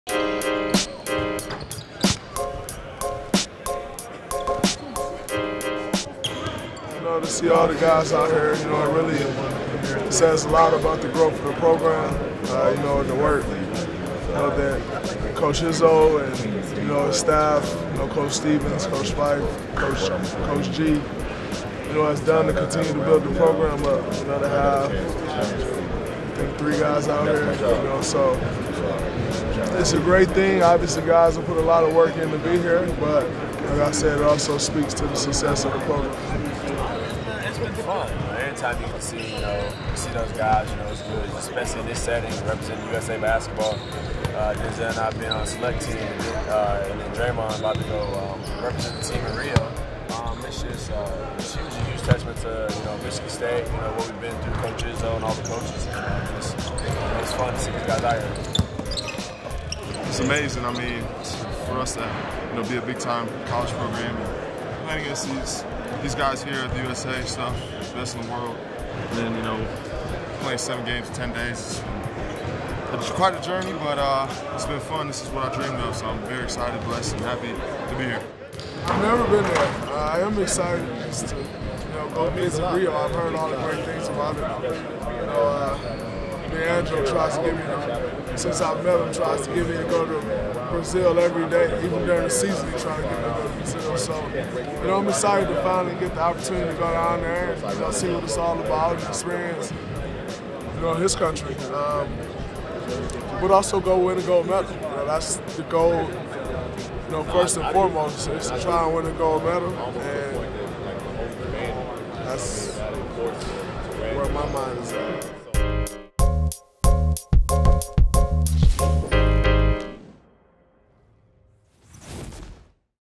Spartans in Las Vegas for USA Basketball
Draymond Green is the third Spartan to represent Team USA at the Summer Olympics, joining Magic Johnson (1992) and Steve Smith (2000).